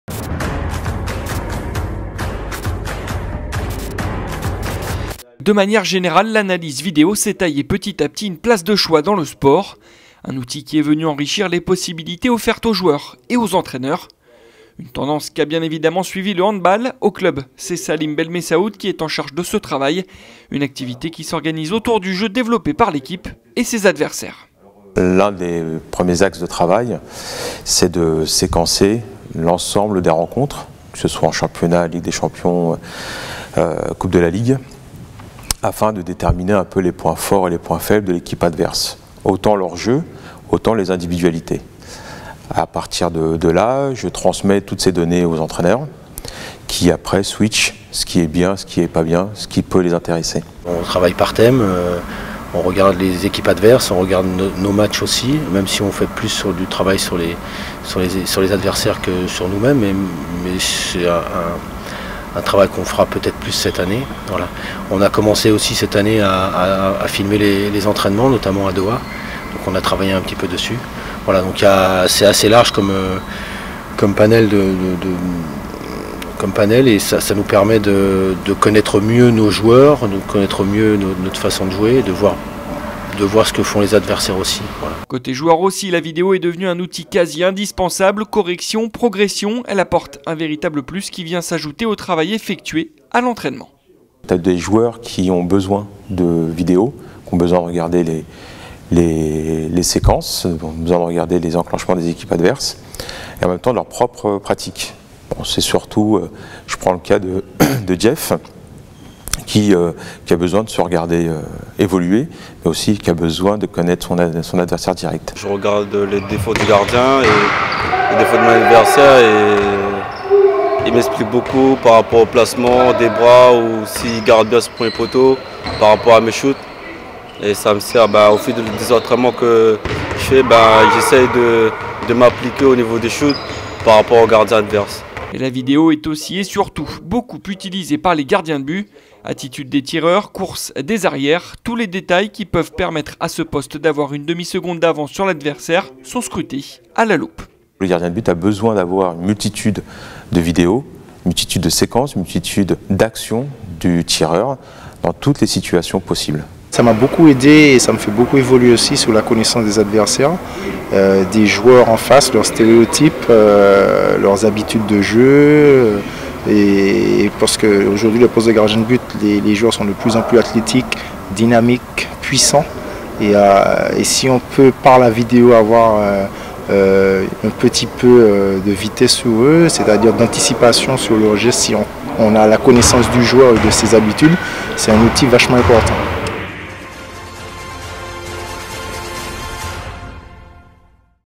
Zoom sur l'utilisation de la vidéo dans le Handball
De manière générale, l’analyse vidéo s’est taillée petit à petit une place de choix dans le sport. Un outil qui est venu enrichir les possibilités offertes aux joueurs et aux entraîneurs.
Une tendance qu’a bien évidemment suivie le Handball. Focus sur son utilisation au sein du Paris Saint-Germain Handball.